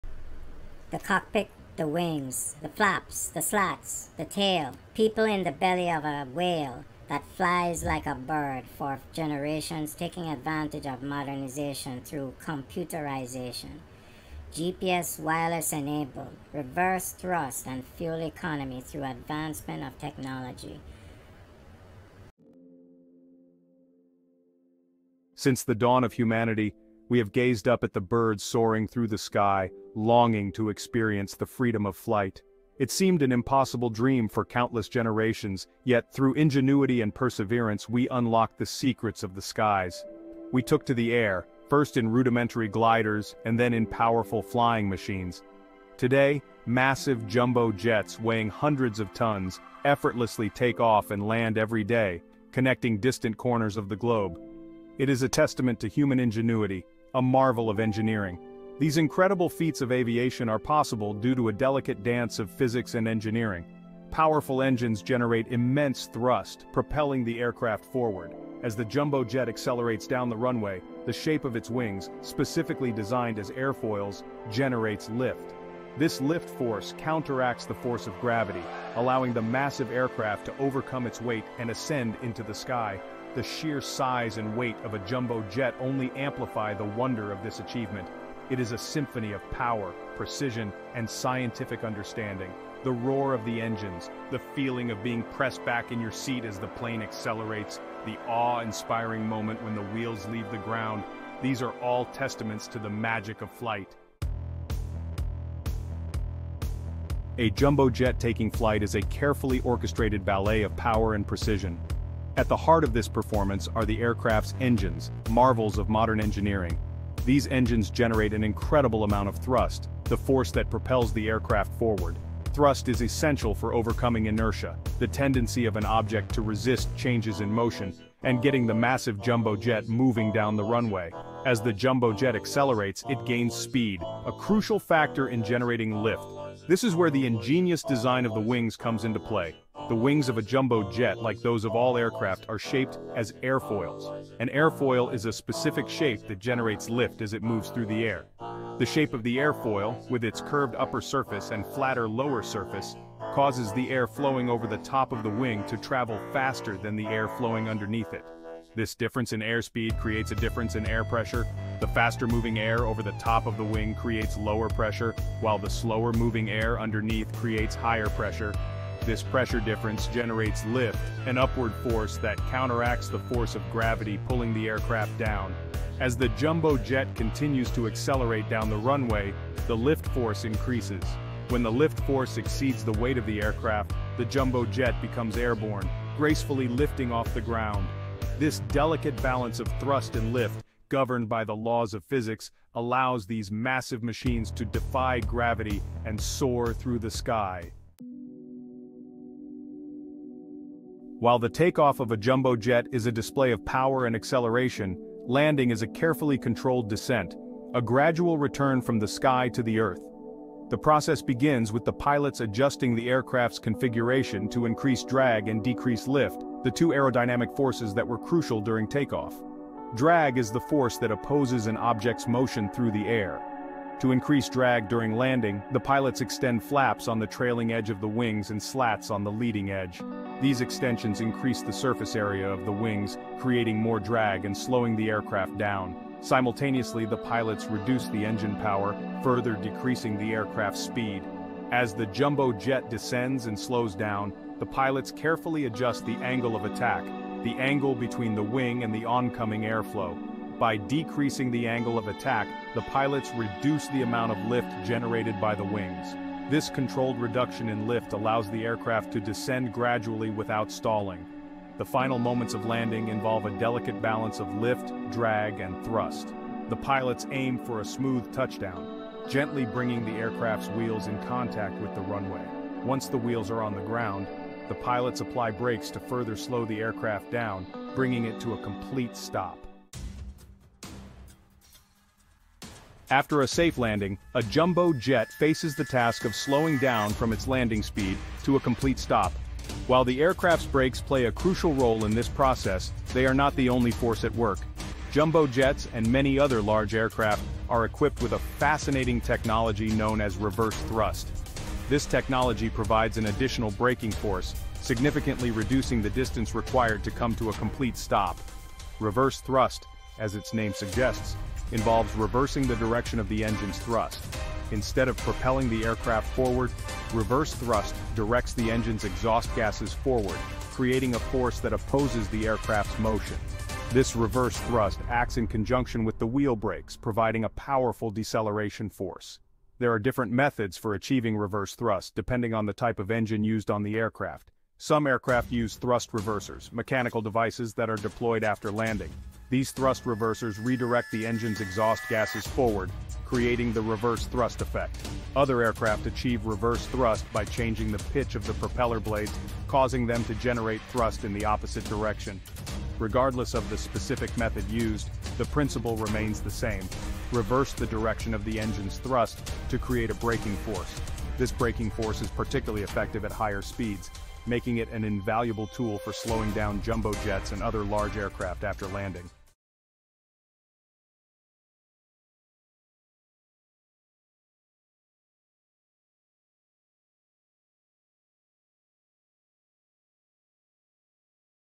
Jumbo Jets Defy Gravity With REVERSE THRUST!
Get ready to have your mind blown as we explore the incredible phenomenon of jumbo jets defying gravity with reverse thrust! From the massive Boeing 747 to the Airbus A380, we'll take a close look at how these commercial airliners use reverse thrust to slow down during landing and takeoff. You'll be amazed at the sheer power and technology that goes into making these behemoths of the skies come to a safe stop. So buckle up and join us on this thrilling journey into the world of aviation!